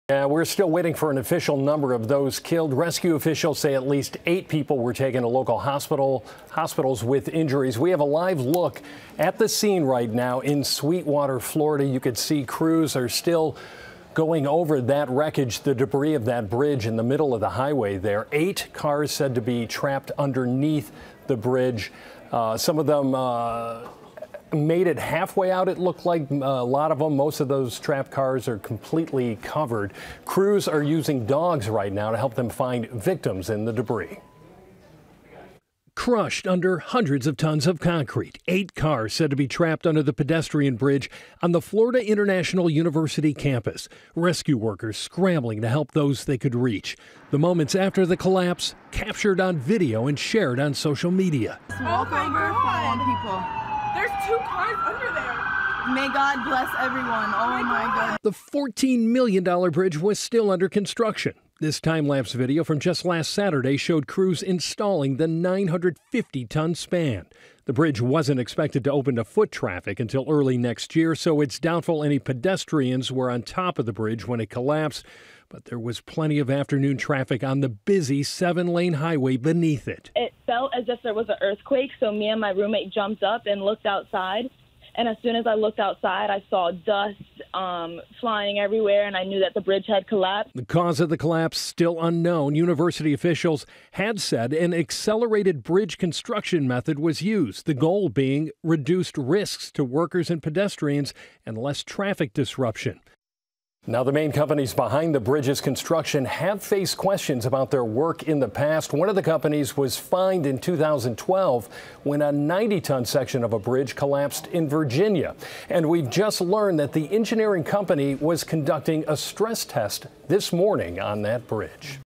Multiple fatalities reported in Florida bridge collapse | ABC7
A number of people died when a newly installed pedestrian bridge on the campus of Florida International University collapsed on Thursday.